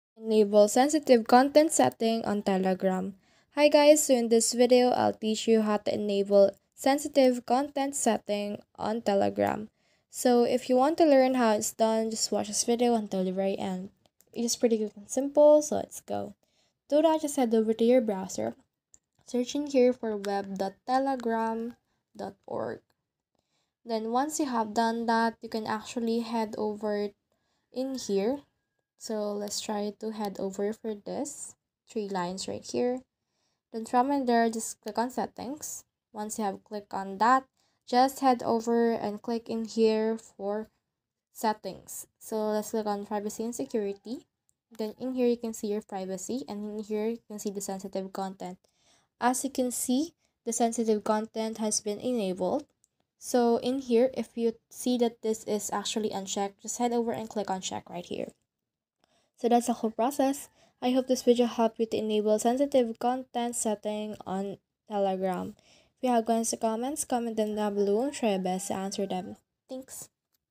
How To Enable Sensitive Content Settings On Telegram 2024 GUIDE
Learn how to enable sensitive content settings on Telegram with this 2024 guide. Ensure a personalized and secure experience on the messaging platform by navigating through the settings menu. Locate the privacy or content settings section where you can enable controls for sensitive content. Follow the prompts to adjust the settings according to your preferences, allowing you to manage and filter sensitive content effectively. Stay updated on the latest features and updates Telegram offers in 2024, enhancing your control over the content you encounter on the platform. This guide empowers you to customize your Telegram experience by enabling sensitive content settings, providing you with the tools to curate your content feed according to your comfort and preferences.